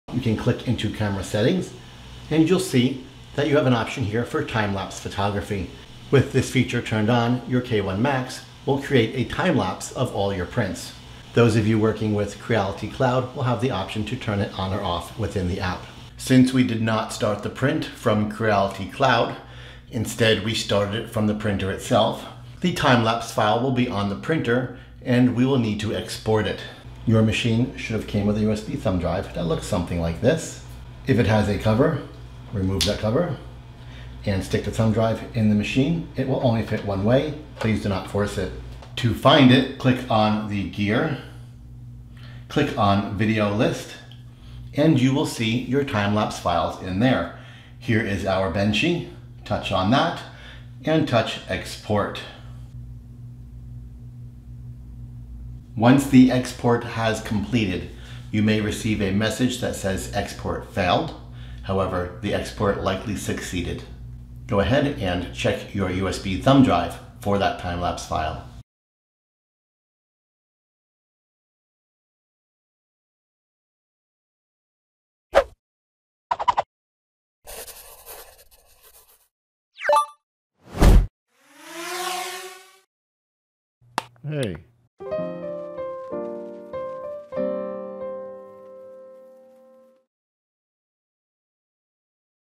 Making & Accessing a Timelapse video on Creality K1, K1C, K1 MAX 3D Printer using USB Thumb Drive.
My 3D Printing Udemy Course:
3DRUNDOWN.COM
CURARUNDOWN.COM
BAMBURUNDOWN.COM